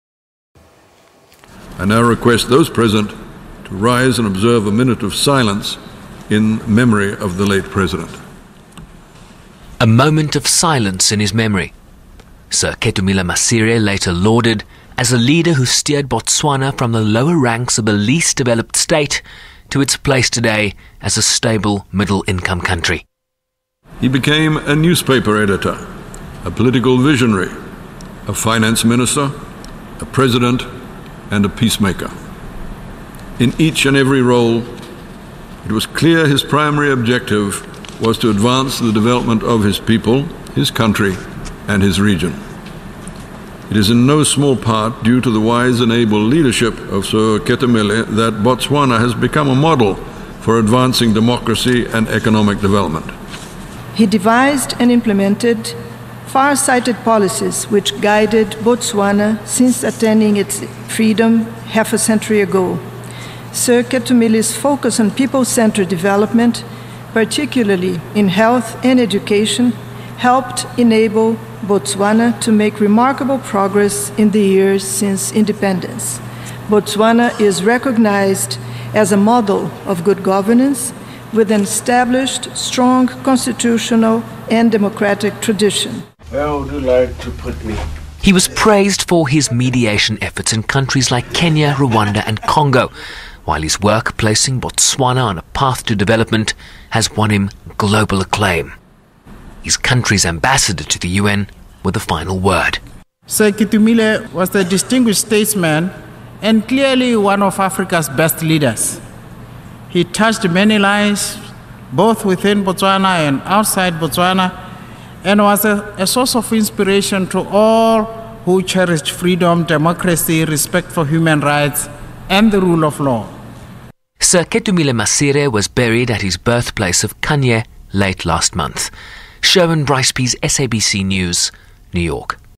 UN pays tribute to Sir Ketumile Masire
The United Nations General Assembly has paid tribute to Botswana's late President, Sir Ketumile Masire.
He died last month at the age of 91.
The United Nations Secretariat and Regional Groupings lauded his example as a democrat, statesman, visionary and peacemaker in Africa.